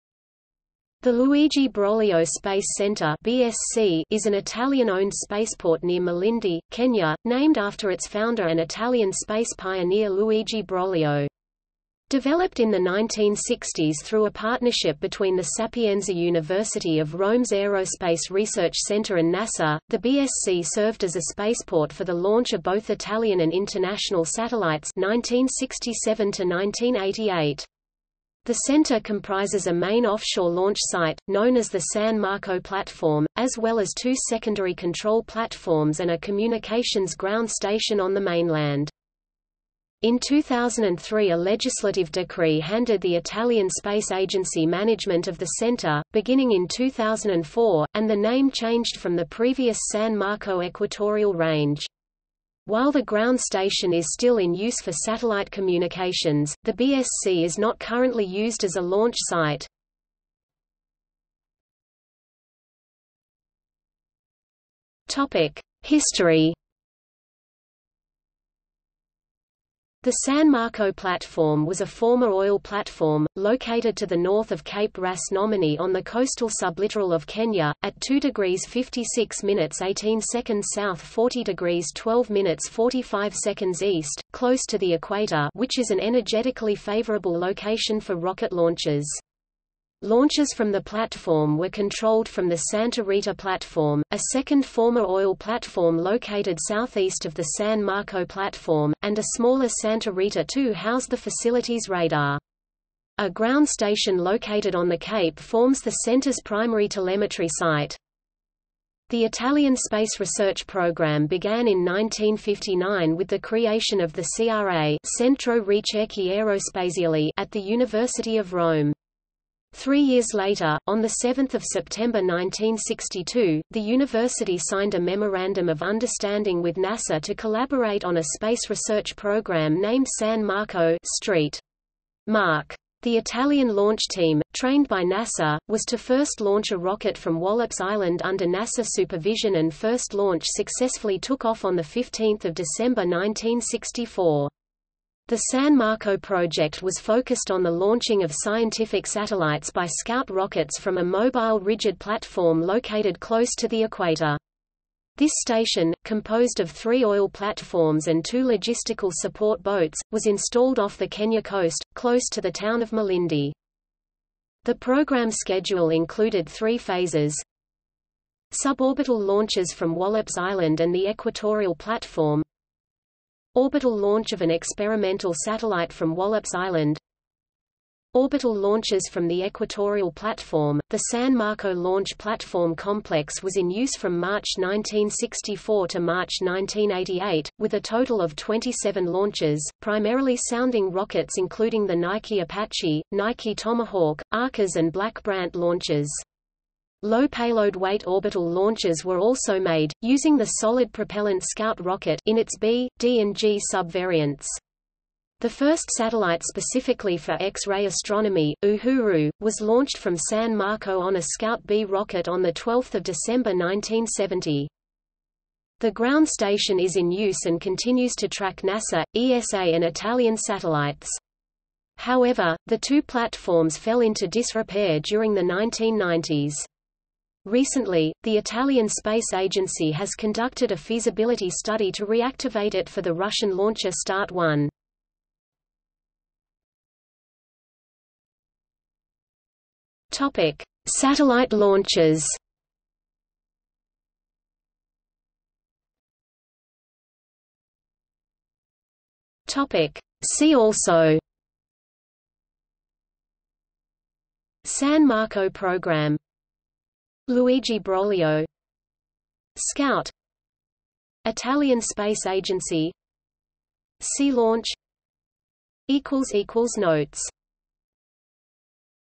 Broglio Space Centre | Wikipedia audio article
00:01:01 1 History
00:03:56 2 Satellite launches
00:04:06 3 See also

- Socrates

SUMMARY
The Luigi Broglio Space Center (BSC) is an Italian-owned spaceport near Malindi, Kenya, named after its founder and Italian space pioneer Luigi Broglio. Developed in the 1960s through a partnership between the Sapienza University of Rome's Aerospace Research Centre and NASA, the BSC served as a spaceport for the launch of both Italian and international satellites (1967–1988).  The center comprises a main offshore launch site, known as the San Marco platform, as well as two secondary control platforms and a communications ground station on the mainland.
In 2003 a legislative decree handed the Italian Space Agency management of the center, beginning in 2004, and the name changed from the previous San Marco Equatorial Range. While the ground station is still in use for satellite communications, the BSC is not currently used as a launch site.